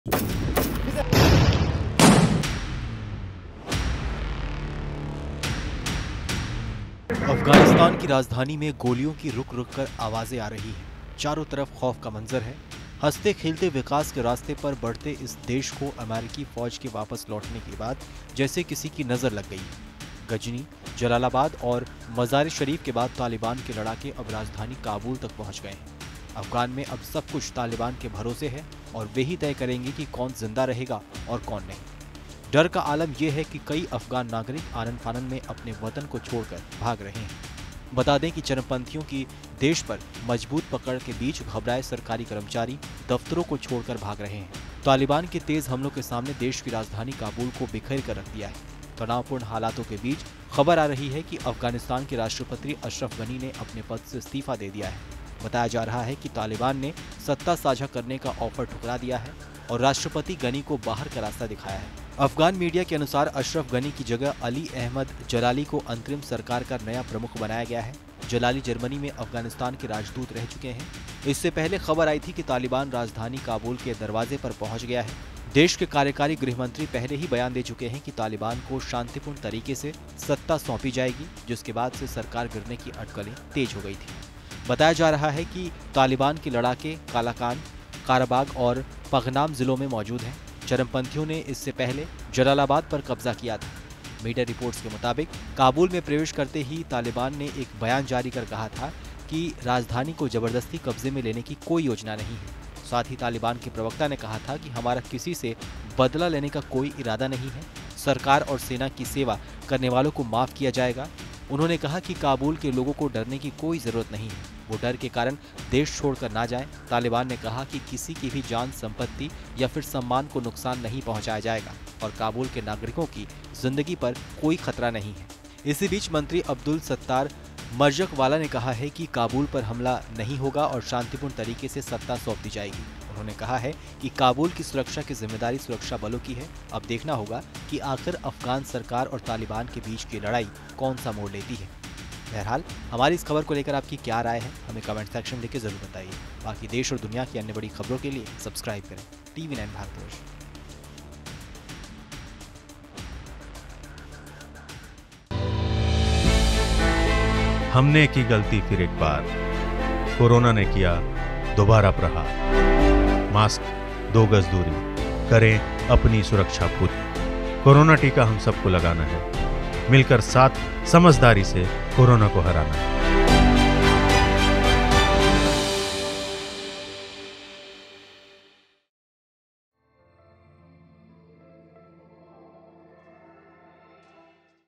Afghanistan में फिर लौटा Taliban युग, तालिबान के आगे अफगानिस्तान सरकार ने टेके घुटने
अफगान की राजधानी में गोलियों की रूक-रूक कर आवाजें आ रही है. अफगान में अब सब कुछ तालिबान के भरोसे है और वे ही तय करेंगे कि कौन जिंदा रहेगा और कौन नहीं. डर का आलम ये है कि कई अफगान नागरिक आनन-फानन में अपने वतन को छोड़कर भाग रहे हैं.

Credit: International । Arti Yadav/Producer । Ajeet/Editor ।